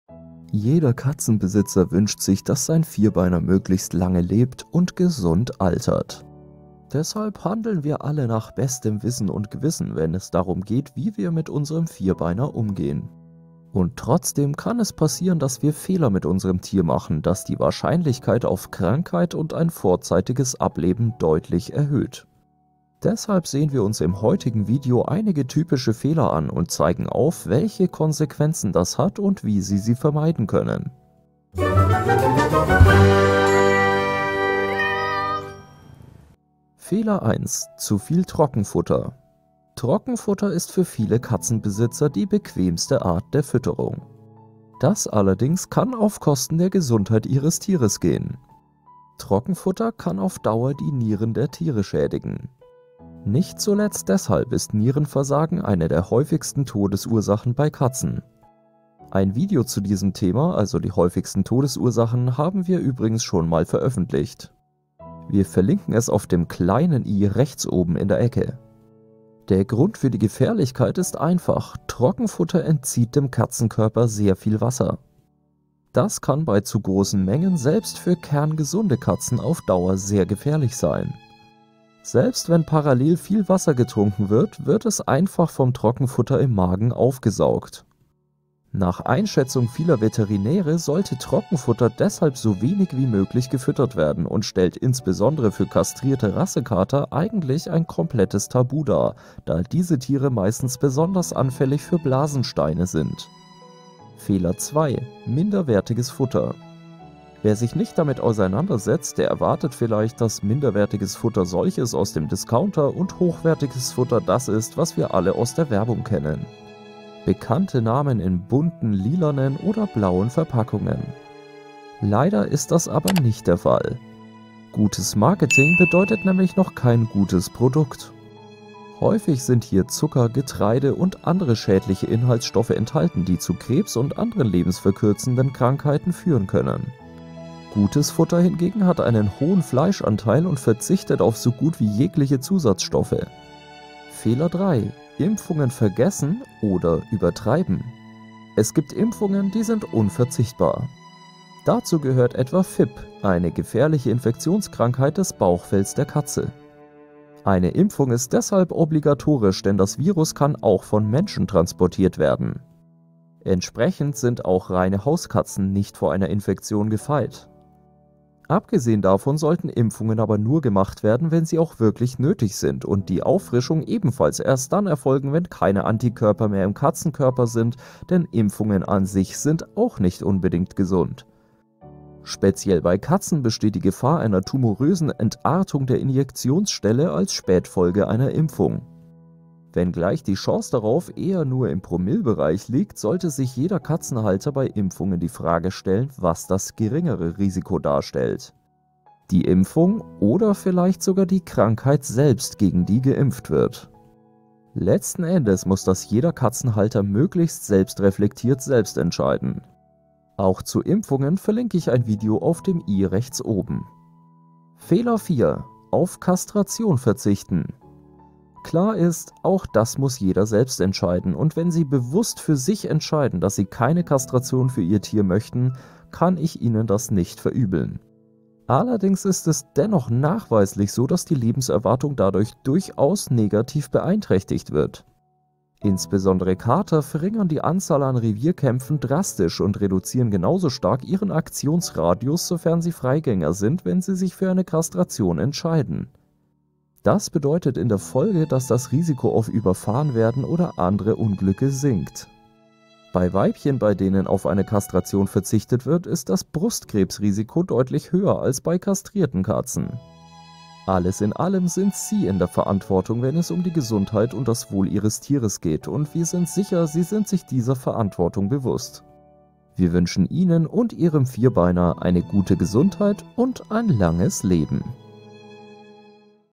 DAS MACHT IHRE KATZE KRANK - Schlimme Fehler, die ein Katzenleben verkürzen
Jeder Katzenbesitzer wünscht sich, dass sein Vierbeiner möglichst lange lebt und gesund altert. Deshalb handeln wir alle nach bestem Wissen und Gewissen, wenn es darum geht, wie wir mit unserem Vierbeiner umgehen. Und trotzdem kann es passieren, dass wir Fehler mit unserem Tier machen, dass die Wahrscheinlichkeit auf Krankheit und ein vorzeitiges Ableben deutlich erhöht. Deshalb sehen wir uns in diesem Video einige typische Fehler an und zeigen auf, welche Konsequenzen das hat und wie sie vermieden werden können.